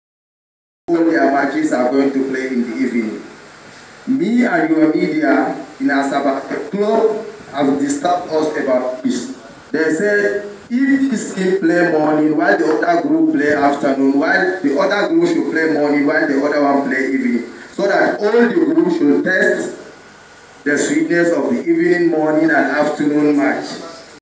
Observation from the Journalist Concerned the Draw Skeletal Format, NNL Relegation PlayOFF
Observation from the Journalist Concerned the Draw Skeletal Format, NNL Relegation PlayOFf in Kano2023 .